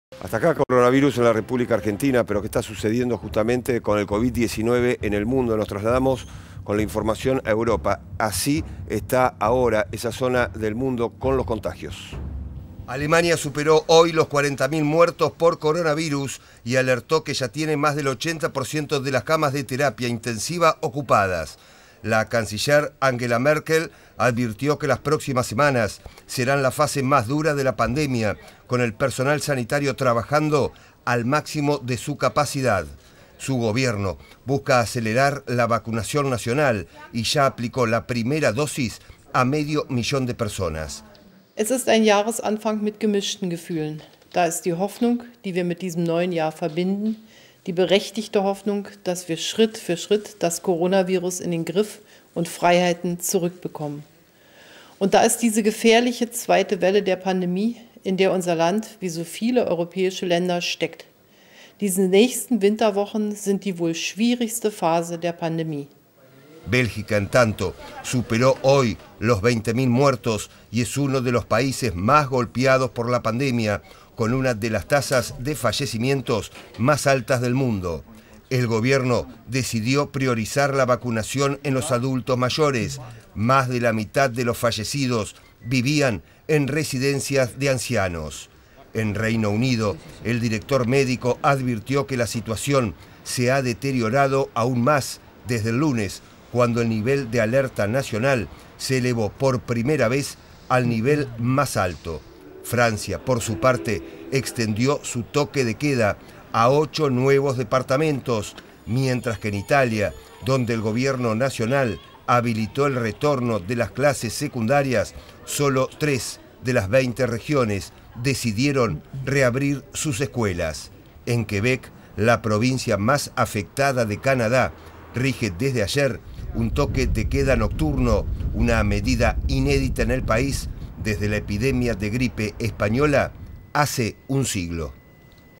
El coronavirus en Europa no cede
En Alemania, la canciller Angela Merkel cree que viene lo peor. Bélgica superó los 20.000 muertos. Reino Unido, Francia e Italia, en alerta es máxima. La situación en Estados Unidos y Canadá.